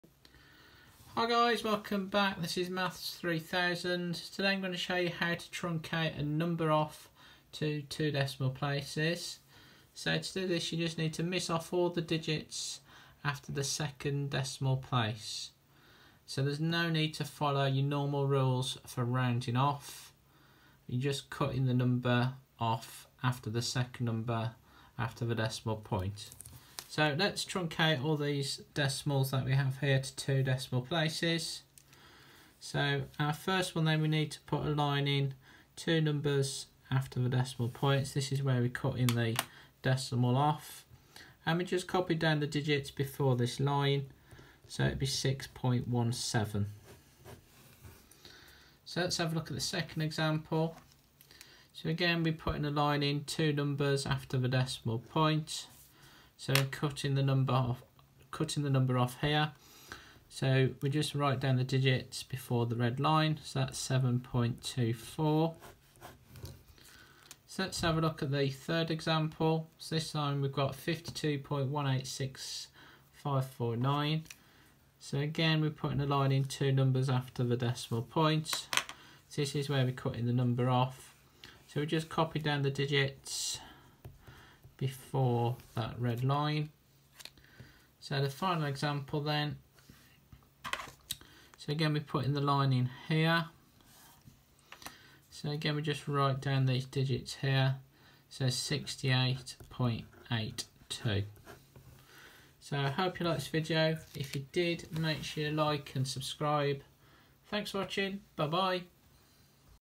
Truncation to 2 Decimal Places
In this video you will be shown how to truncate a number to 2 decimal places. To do this miss all the digits off after the second decimal place. Don’t follow the normal rules for rounding.

In example 1, you are asked to truncate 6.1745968 to 2 decimal places. Put a line in 2 numbers after the decimal point (after the hundredths column). Copy down all the digits before the line to give 6.17.

In example 3 you are asked to truncate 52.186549 to 2 decimal places. Again miss all the digits off two places after the decimal point to give 52.18.